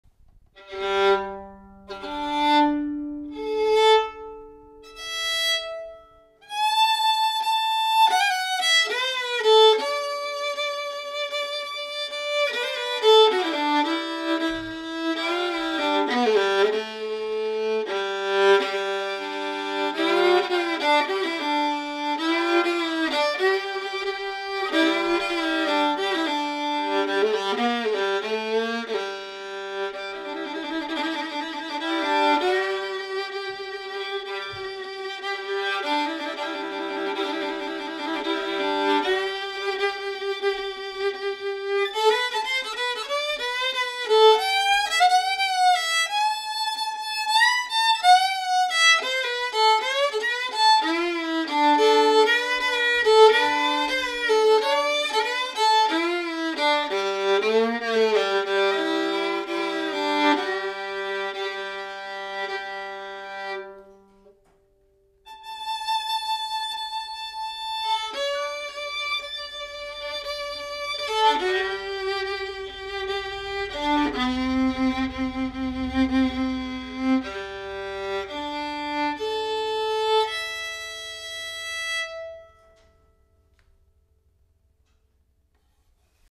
1910s German-made 4/4 Strad-copy violin
This is a “Strad copy” 4/4 violin, probably made in Germany around 1910, and in good order. Andy did some setup work and board leveling work a few weeks ago and it’s playing spot-on and has a sweet, husky, fiddle-centric sound to it that adores double-stops.